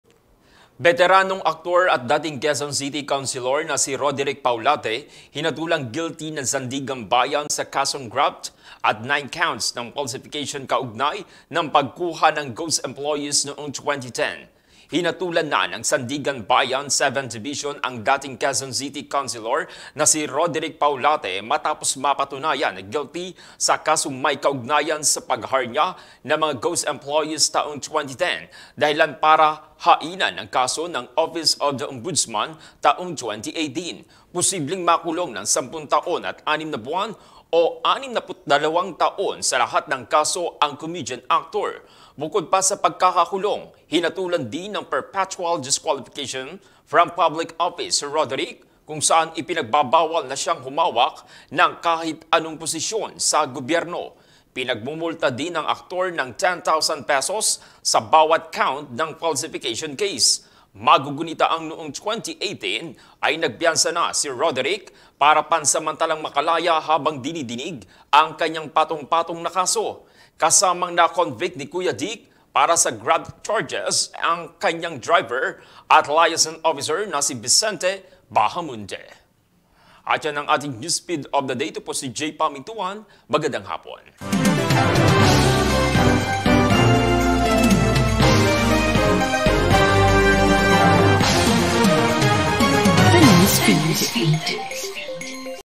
Beteranong aktor at Dating Quezon City councilor na si Roderick Paulate, makukulong ng 62 taon
Beteranong aktor at Dating Quezon City councilor na si Roderick Paulate, hinatulang guilty ng Sandiganbayan sa kasong graft at 9 counts ng falsification kaugnay ng pagkuha ng ghost employees noong 2010.